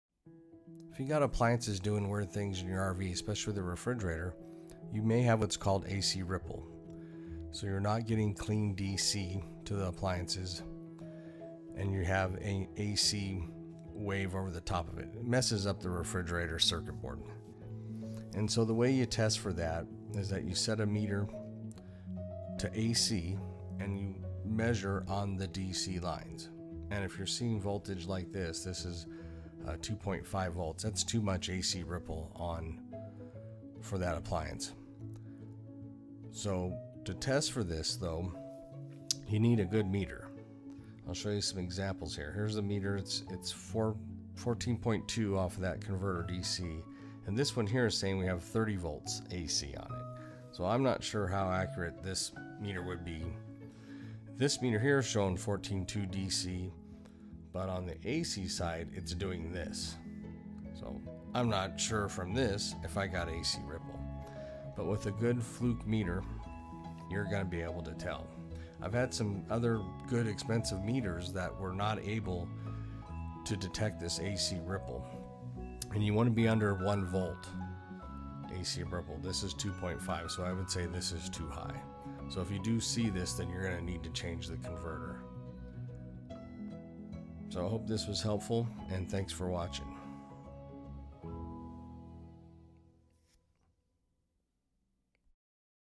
RV Refrigerated Not Working. Check For AC Ripple
I’ve been working on a lot of older RVs and when we get a refrigerator that’s not working testing for AC ripple is one of our 1st tests. It’s easy with the correct meter. Testing for the correct DC voltage is important and while doing that you can test for AC ripple. If it’s higher than 1 volt the converter should be replaced.